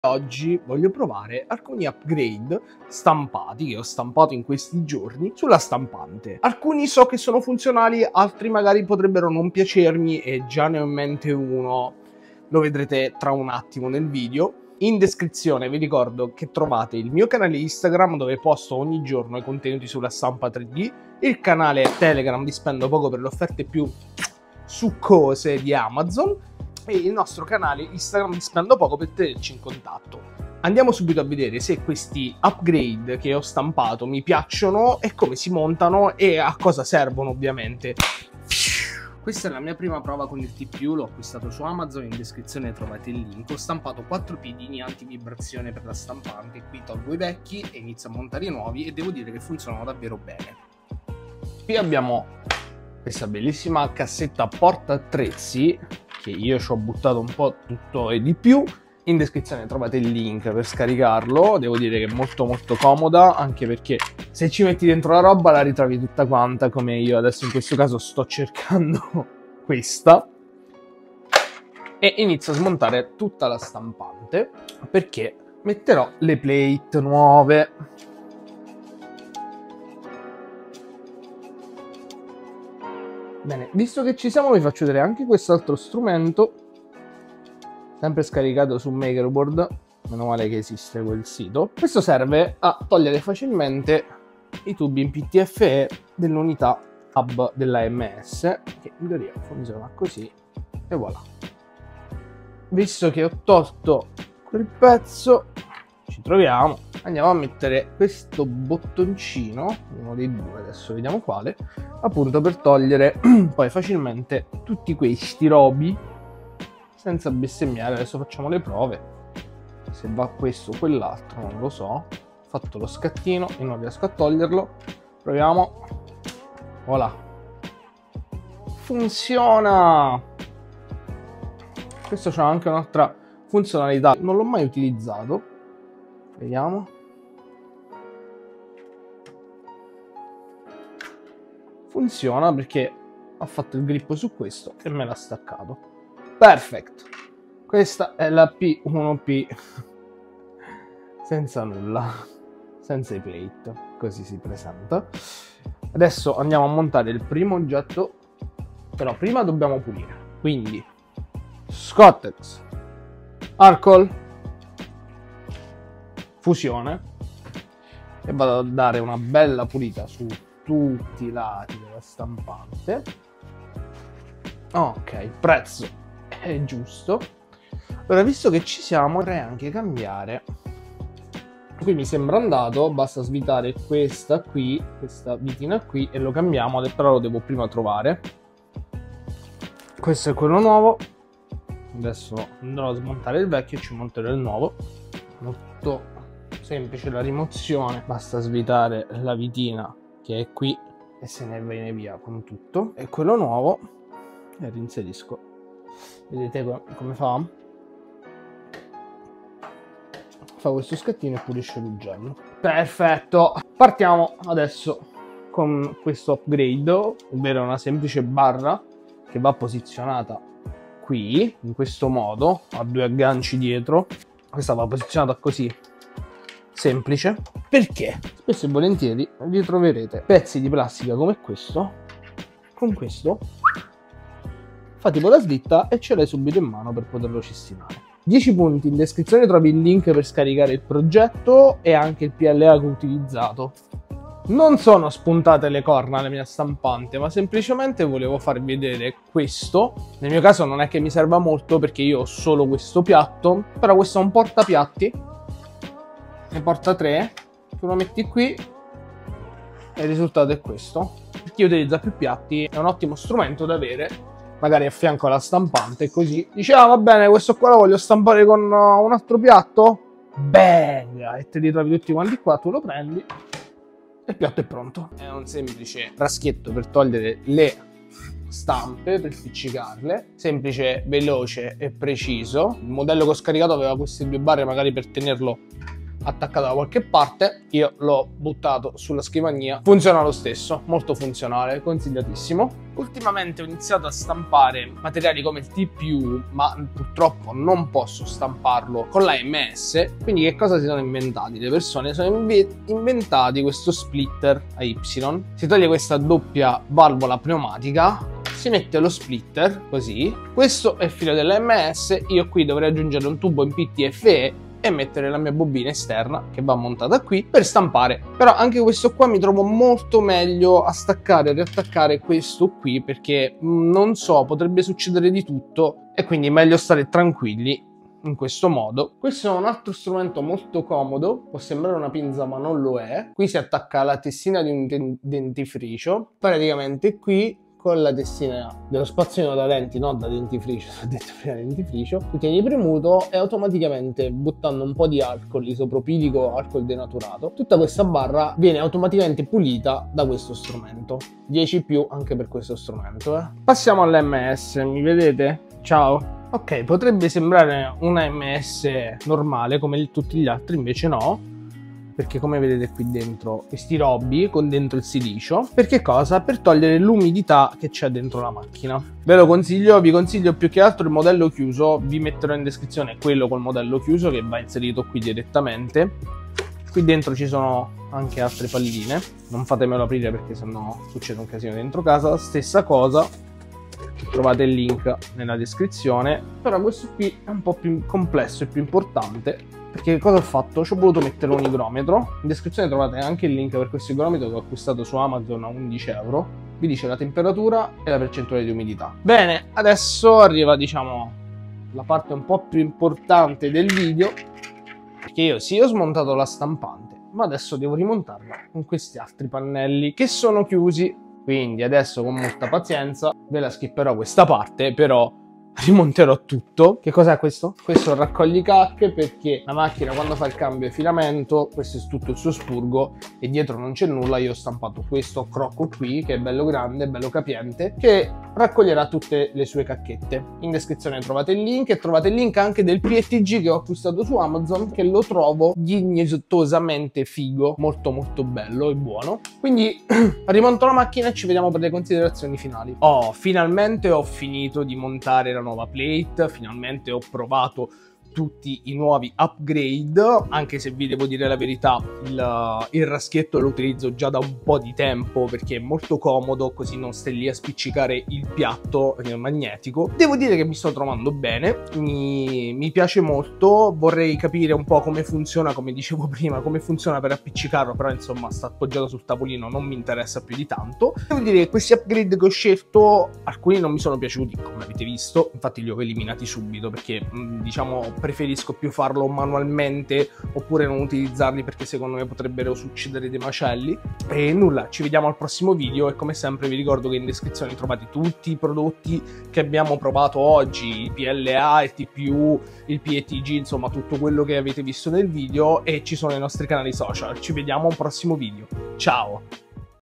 Questi upgrade sono F A V O L O S I (alcuni) 😈
Quali sono gli upgrade che stai più desiderando provare sulla tua stampante 3D? Condividi i tuoi pensieri nei commenti e non dimenticare di iscriverti per altri entusiasmanti video sulla stampa 3D! 🚀✨

0:37 TPU FEET
0:53 CASSETTA PORTATTREZZI
1:25 STRUMENTO AMS
1:49 AMS BUTTON
3:37 BASE SPOILER
4:00 PLA GIALLO
4:05 PLATE HOLDER
4:35 SD SCRAPER
4:55 Y SPLITTER
5:29 ROD CLEANER
5:54 AMS
6:10 AMS FRONT
6:30 IGROMETRO XIAOMI
6:38 AMS XIAOMI
6:59 RACCOGLISPURGO